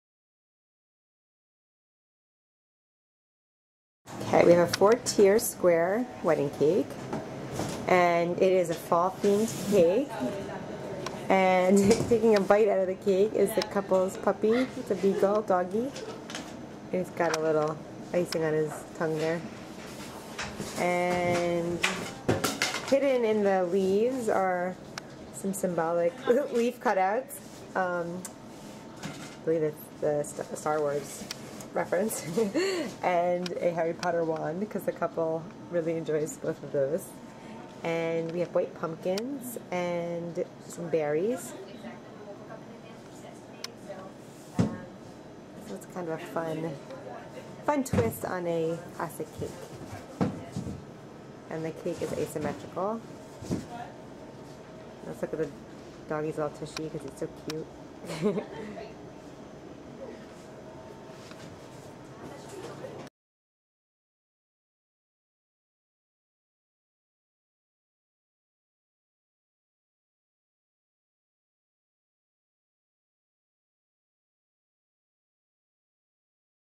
Fall Themed Wedding Caken with Beagle Surprise!
The highlight of this asymmetrically stacked 4 tier wedding cake is the small sculpted beagle named Buddy taking a bite out of the cake. Buddy is looking over slyly at the cake as a small amount of icing and cake rests on his tongue.

White pumpkins, orange, red, and yellow bittersweet berries and cascading fall leaves highlight the rest of the cakes decorations. The entire cake is covered in ivory fondant with shimmery deep red bands around the base of each tier.

Flavors and fillings matched the cakes fall theme and included apple spice with caramel buttercream and chocolate with raspberry chambord and chocolate.